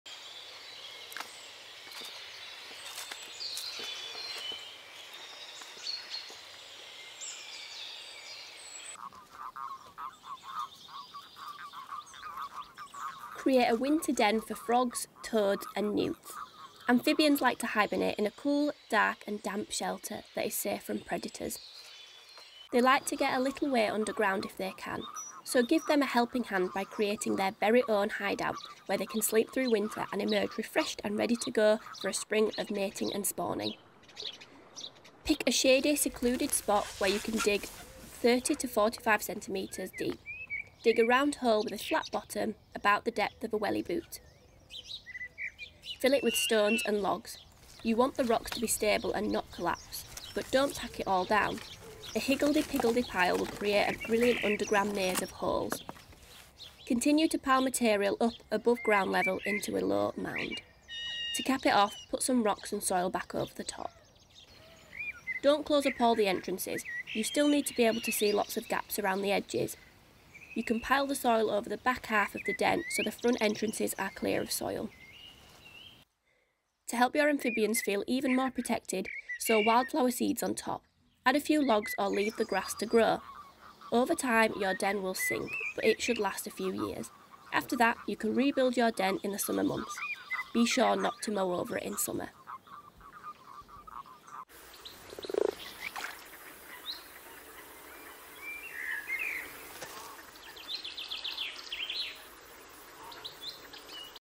How to make a toad and frog house | RSPB Nature on Your Doorstep
Create a winter den for frogs, toads and newts to sleep through winter and emerge refreshed and ready to go for a spring of mating and spawning.